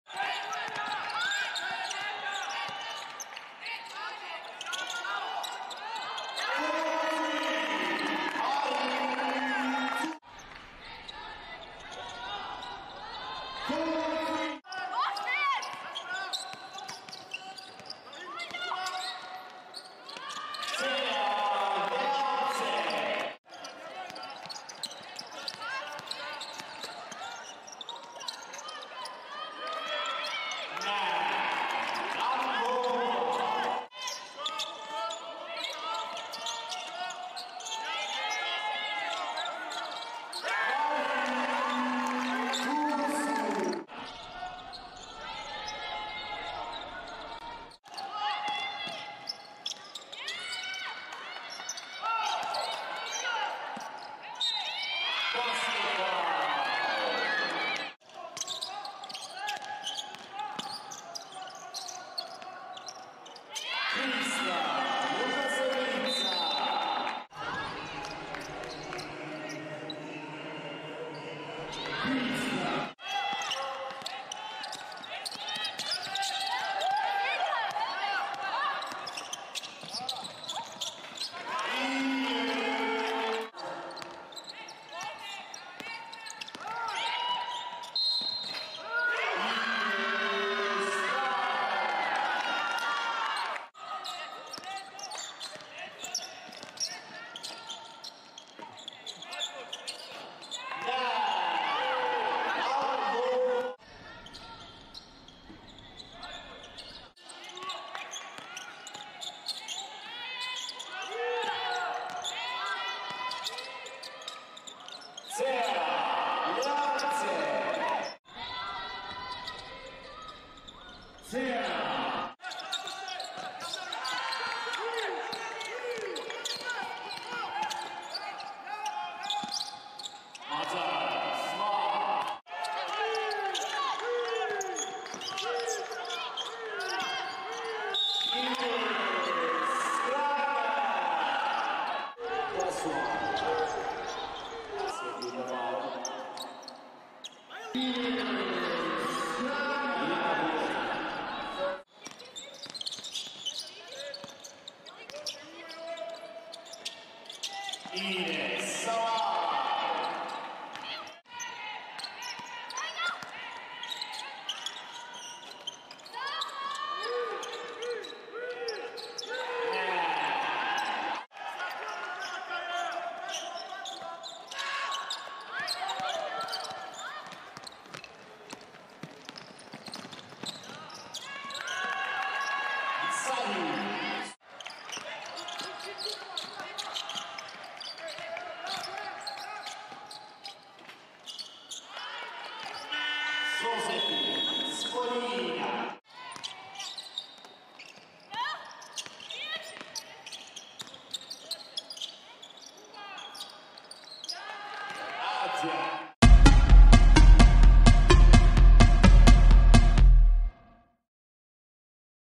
France 🇫🇷 vs Latvia 🇱🇻 | Quarter-Finals Highlights | FIBA U18 Women's European Championship 2023
Watch the highlights between France and Latvia in the Quater-Finals of the FIBA U18 Women's European Championship 2023!